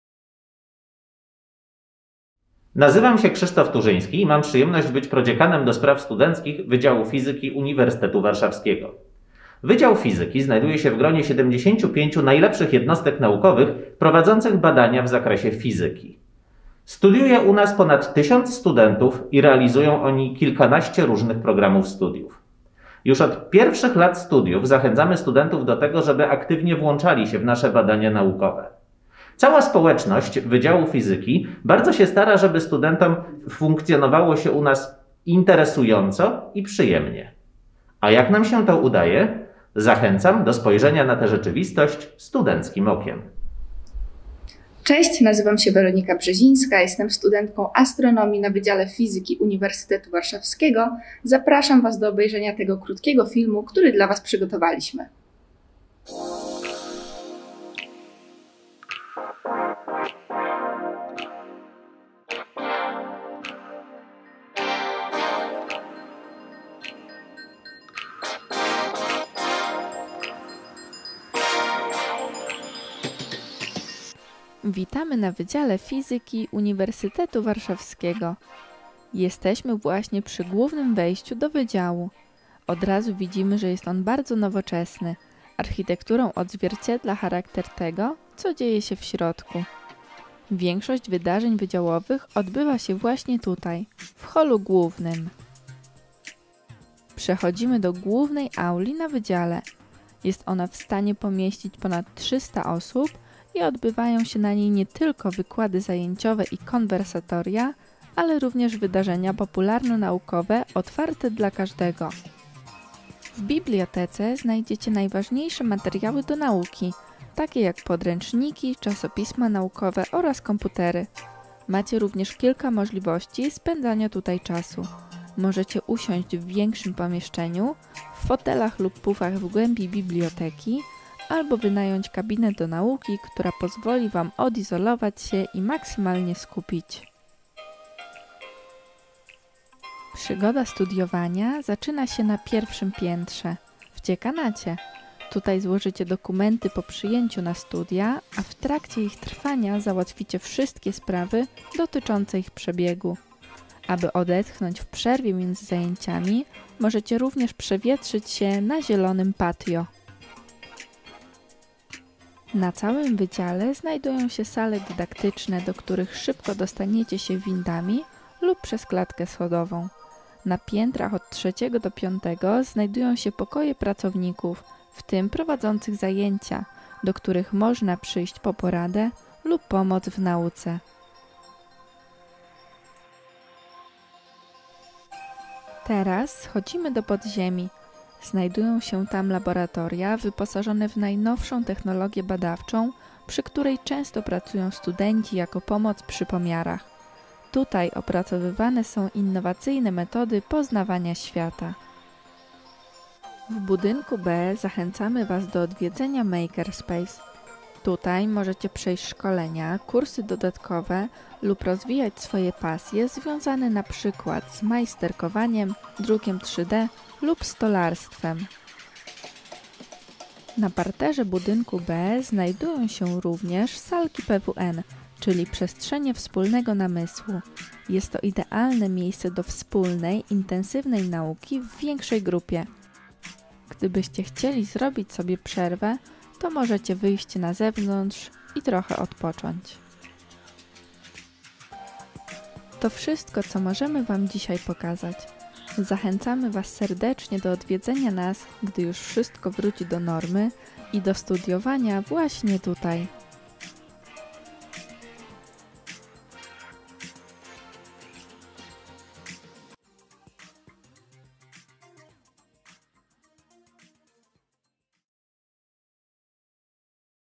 Wydział Fizyki UW "studenckim okiem"
Film o Wydziale Fizyki przygotowany przez Samorząd Studentów FUW